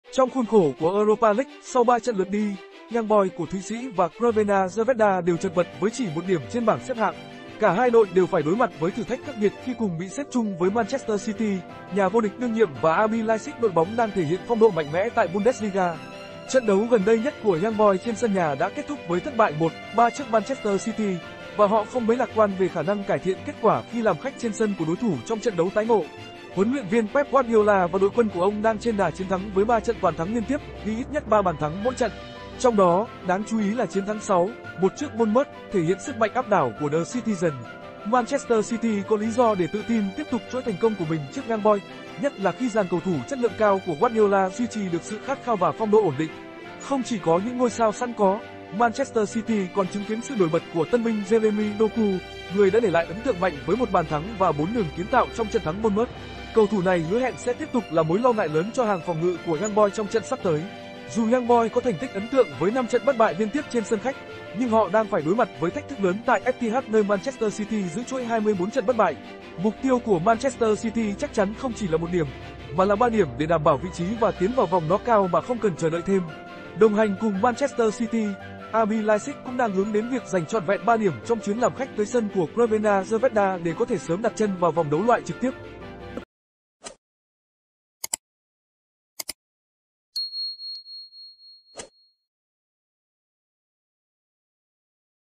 EUROPA LEAGUE | YOUNG BOYS VÀ CRVENA ZVEZDA - ĐỐI ĐẦU MAY RỦI CÙNG BẢNG ĐẤU KHỐC LIỆT
Trong khuôn khổ của Europa League, sau ba trận lượt đi, Young Boys của Thụy Sĩ và Crvena Zvezda đều chật vật với chỉ một điểm trên bảng xếp hạng. Cả hai đội đều phải đối mặt với thử thách khắc nghiệt khi cùng bị xếp chung với  Manchester City, nhà vô địch đương nhiệm và RB Leipzig - đội bóng đang thể hiện phong độ mạnh mẽ tại Bundesliga. Trận đấu gần đây nhất của Young Boys trên sân nhà đã kết thúc với thất bại 1-3 trước  Manchester City, và họ không mấy lạc quan về khả năng cải thiện kết quả khi làm khách trên sân của đối thủ trong trận đấu tái ngộ.  Huấn luyện viên Pep Guardiola và đội quân của ông đang trên đà chiến thắng với ba trận toàn thắng liên tiếp, ghi ít nhất ba bàn thắng mỗi trận. Trong đó, đáng chú ý là chiến thắng 6-1 trước Bournemouth, thể hiện sức mạnh áp đảo của The Citizens.  Manchester City có lý do để tự tin tiếp tục chuỗi thành công của mình trước Young Boys, nhất là khi dàn cầu thủ chất lượng cao của Guardiola duy trì được sự khát khao và phong độ ổn định. Không chỉ có những ngôi sao sẵn có,  Manchester City còn chứng kiến sự nổi bật của tân binh Jeremy Doku, người đã để lại ấn tượng mạnh với một bàn thắng và bốn đường kiến tạo trong trận thắng Bournemouth. Cầu thủ này hứa hẹn sẽ tiếp tục là mối lo ngại lớn cho hàng phòng ngự của Young Boys trong trận sắp tới. Dù Young Boys có thành tích ấn tượng với năm trận bất bại liên tiếp trên sân khách, nhưng họ đang phải đối mặt với thách thức lớn tại Etihad - nơi  Manchester City giữ chuỗi 24 trận bất bại. Mục tiêu của  Manchester City chắc chắn không chỉ là một điểm, mà là ba điểm để đảm bảo vị trí và tiến vào vòng knock-out mà không cần chờ đợi thêm. Đồng hành cùng  Manchester City, RB Leipzig cũng đang hướng đến việc giành trọn vẹn ba điểm trong chuyến làm khách tới sân của Crvena Zvezda để có thể sớm đặt chân vào vòng đấu loại trực tiếp.